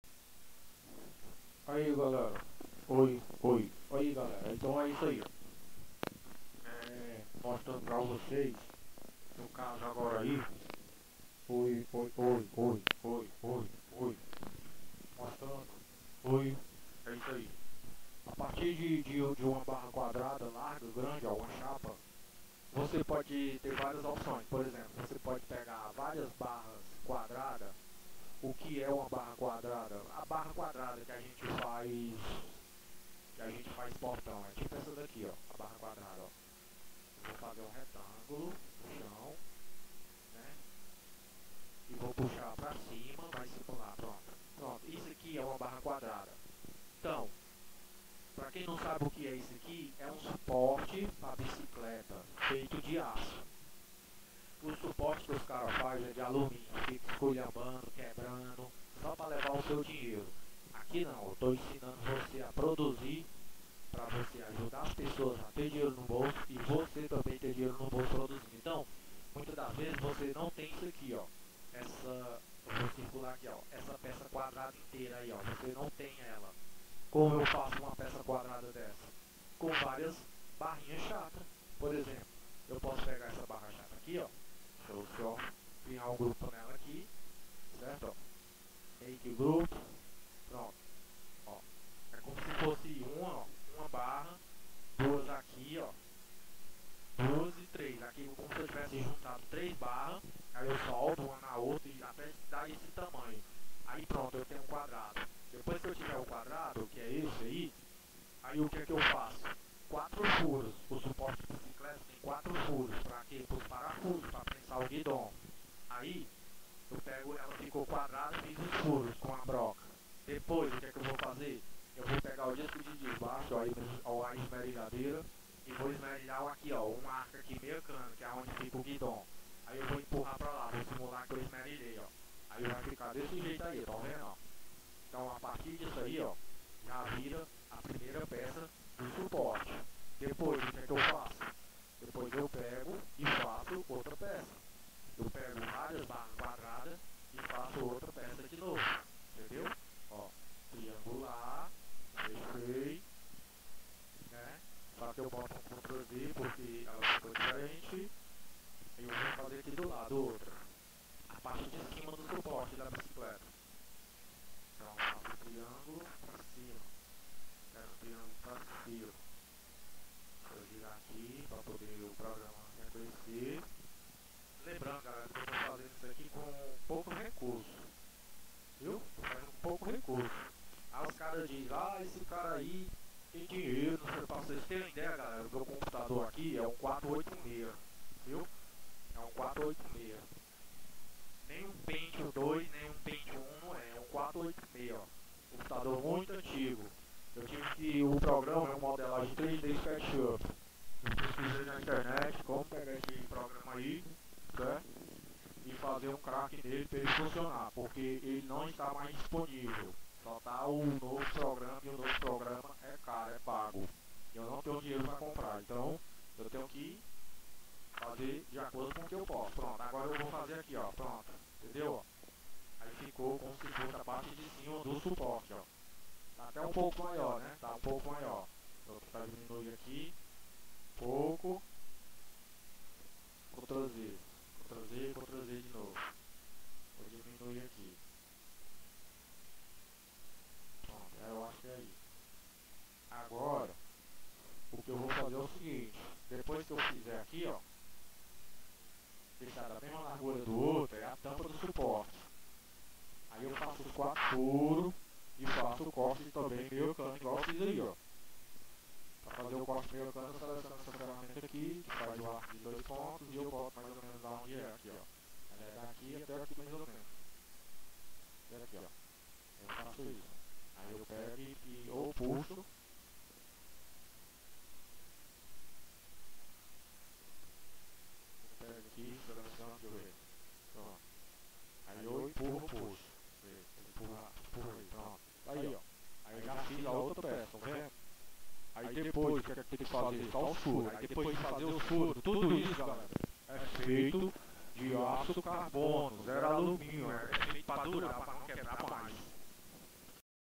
fabricando peças bicicleta de aço feita para durar nao quebrar e bem barato que pobre pode comprar
fabricando peças de bicicleta de aço feita para durar nao quebrar e ser bem barato que pobre pode comprar